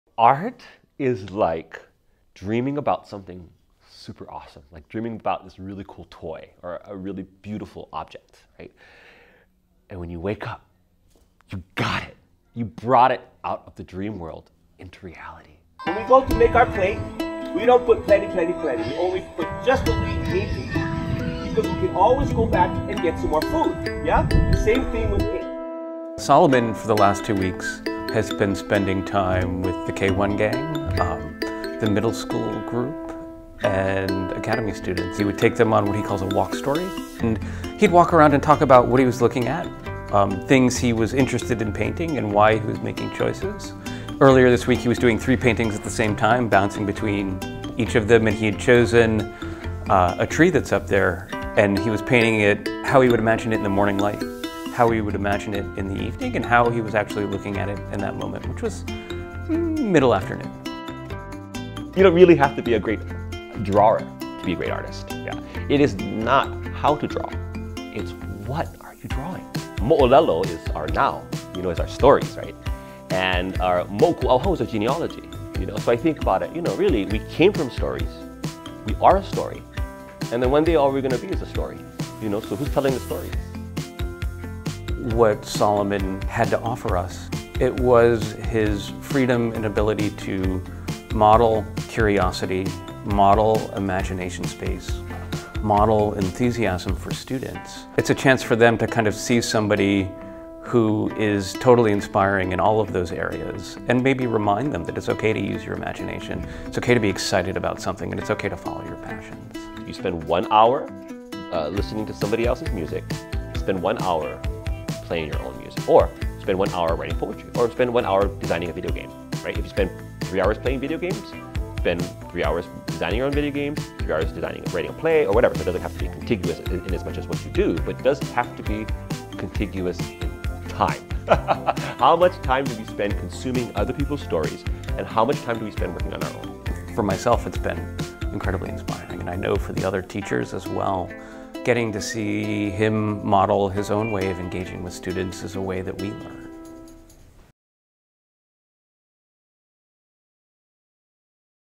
Artist in Residence: Solomon Enos
Wai‘anae artist Solomon Enos visited campus this winter, working with students in the studio, en plein air, and in Bishop Learning Commons and the Academy Learning Commons in Cooke Library.